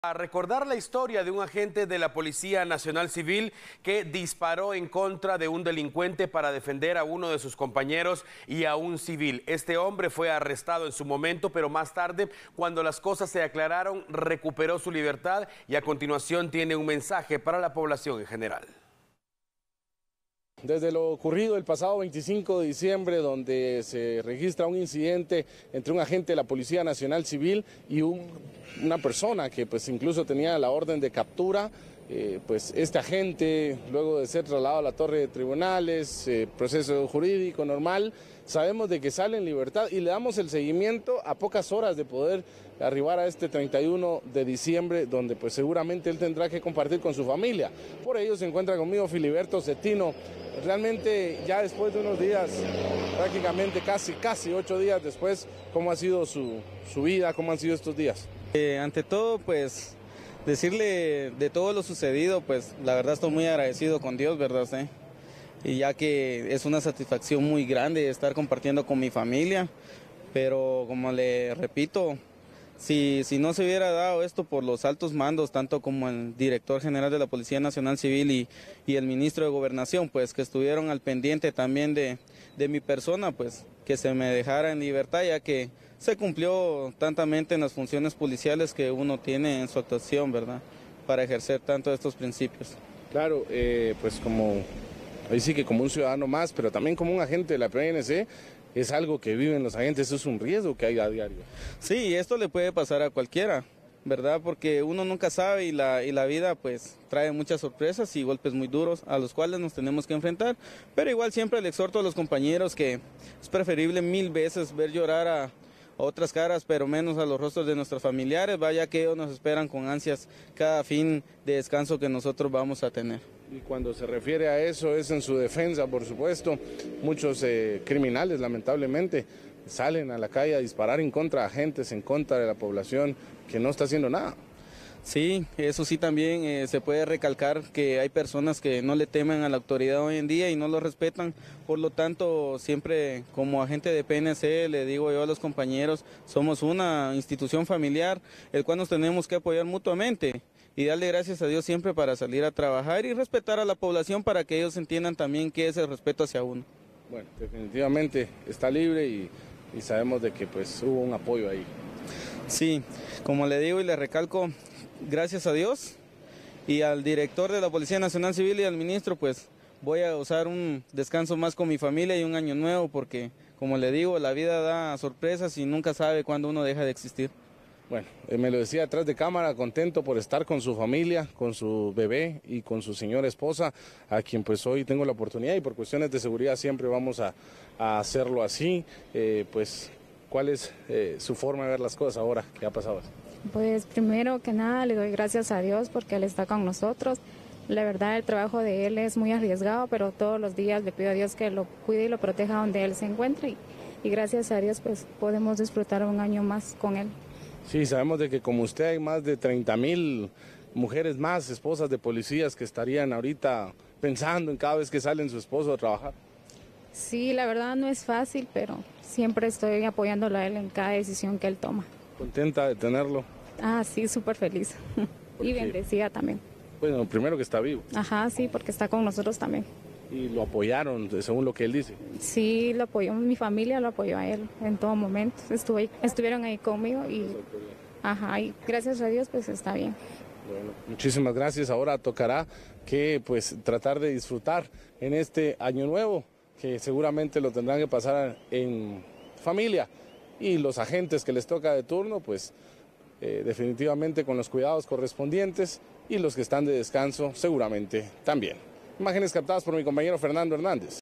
PNC accionó arma en cumplimiento de su deber
El elemento disparó contra peligroso pandillero

El PNC Filiberto Centino envió un mensaje a la población tras recuperar su libertad, luego de haber dado muerte a un peligroso pandillero en un operativo.